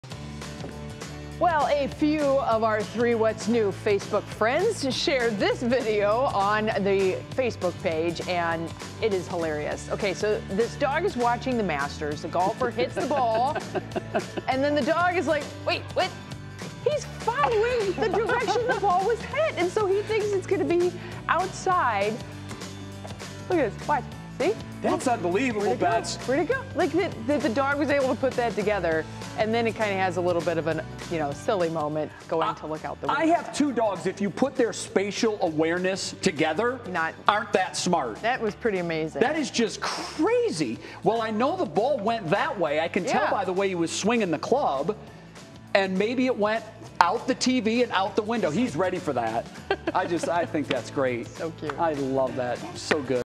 Dog watching golf on tv searches for ball in tonight's Worth the Watch
Check out this cute dog watching golf on TV. A few of our 'What's New' Facebook friends shared his video on our page and its hilarious.

It appears this pup is watching The Masters.

The golfer hits the ball and the dog waits for a second.  The dog then runs to the window to look for the dog.

This dog video is definitely Worth the Watch!